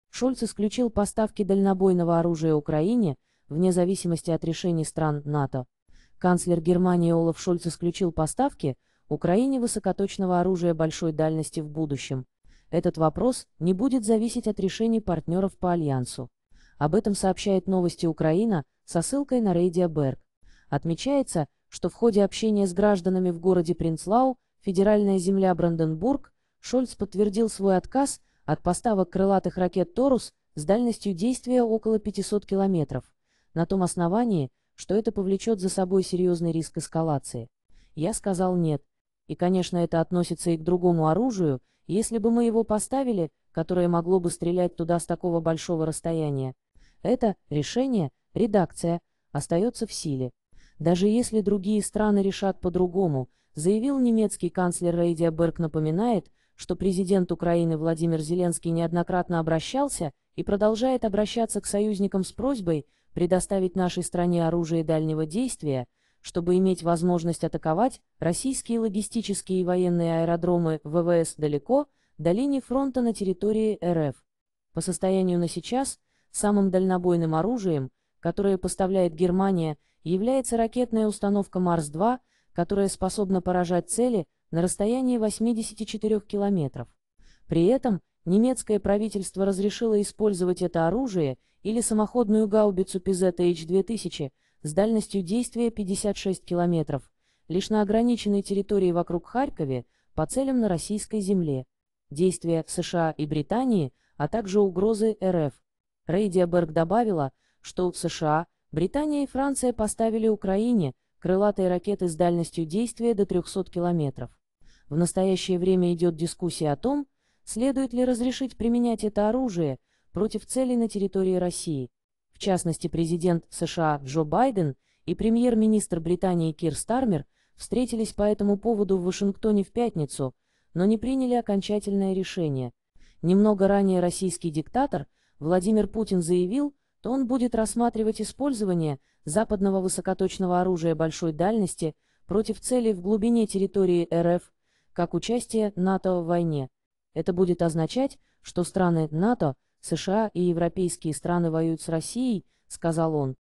Шольц исключил поставки дальнобойного оружия Украине вне зависимости от решений стран НАТО.
РоссийскаяФедерация Германия ВойнавУкраине ОлафШольц Канцлер Германии Олаф Шольц исключил поставки Украине высокоточного оружия большой дальности в будущем. Этот вопрос не будет зависеть от решений партнеров по Альянсу. Об этом сообщает Новости Украина со ссылкой на Radio Berg. Отмечается, что в ходе общения с гражданами в городе Пренцлау (федеральная земля Бранденбург), Шольц подтвердил свой отказ от поставок крылатых ракет Taurus (с дальностью действия около 500 км) на том основании, что это повлечет за собой "серьезный риск эскалации". "Я сказал н